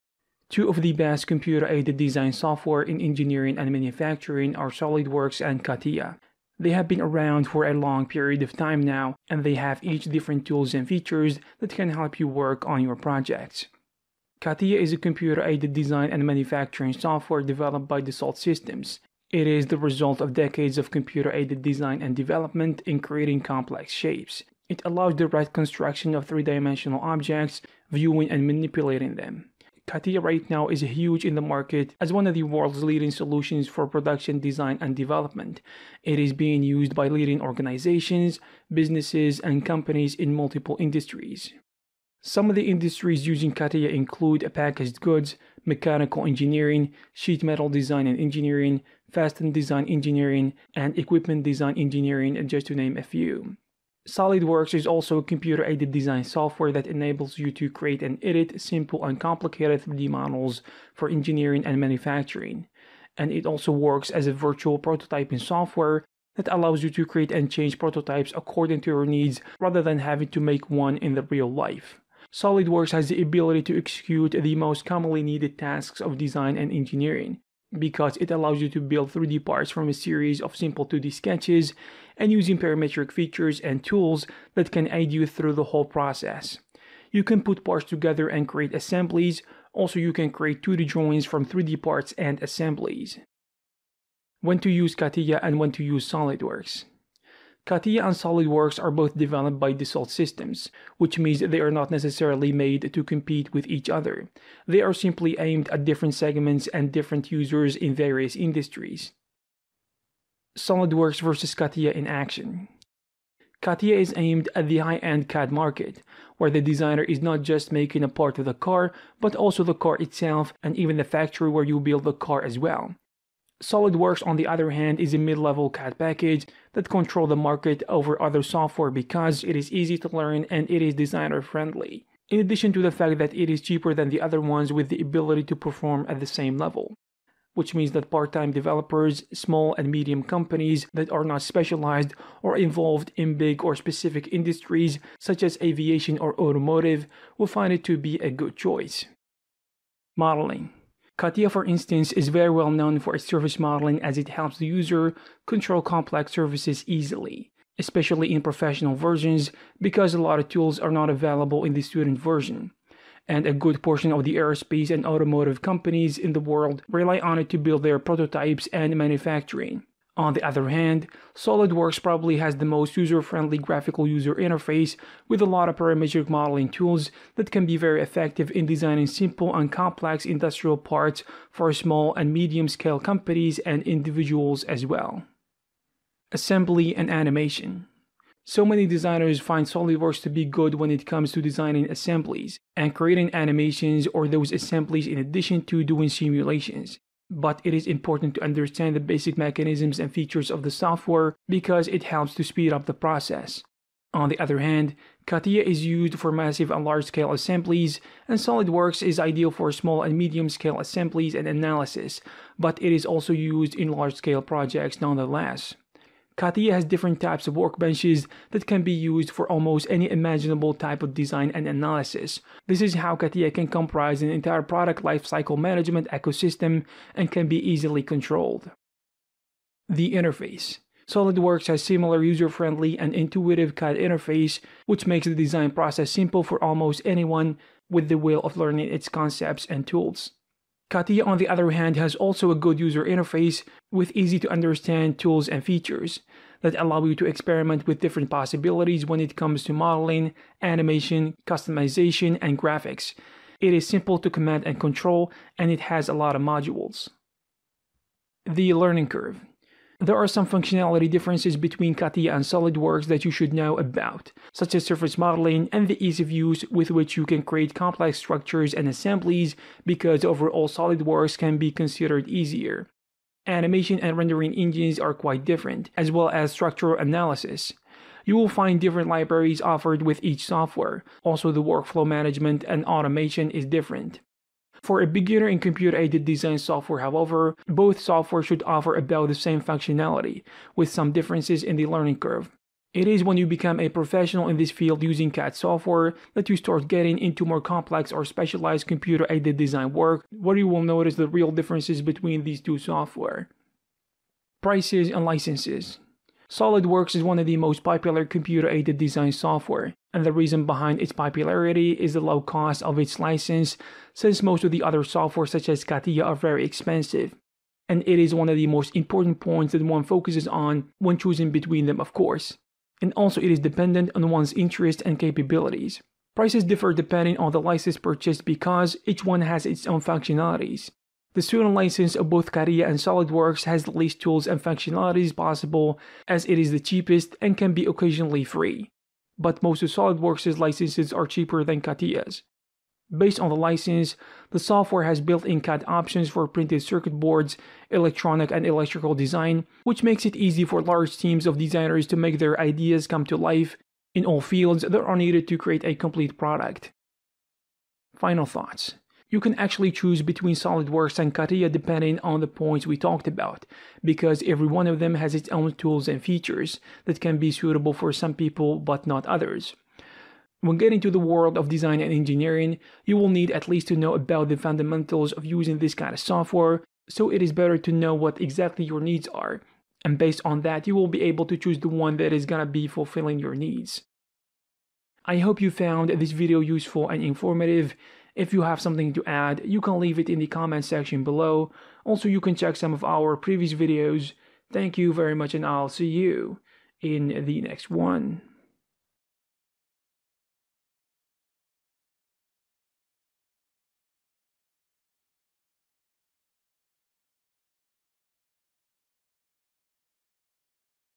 Catia vs solid works which one is Better.
two of the best computer-aided design software in engineering and manufacturing are Solidworks and CATIA, they have been around for a long period of time now, and they each have different tools and features that can help you work on your projects.